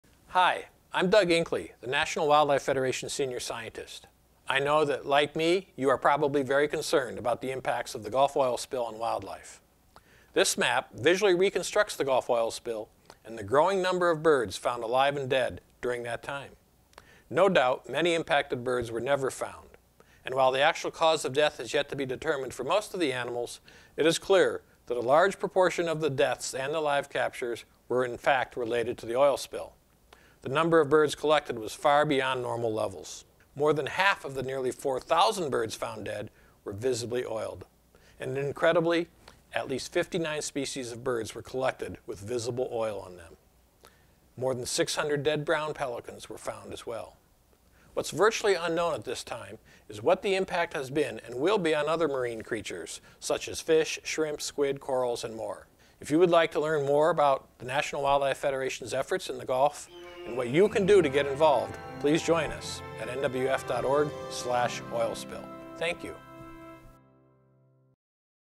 Bird Impact Map- Gulf Oil Spill
NWF Senior Scientist Dr. Dough Inkley discusses bird impacts from the Gulf oil spill.

National Wildlife Federation works on protecting wildlife and wild places.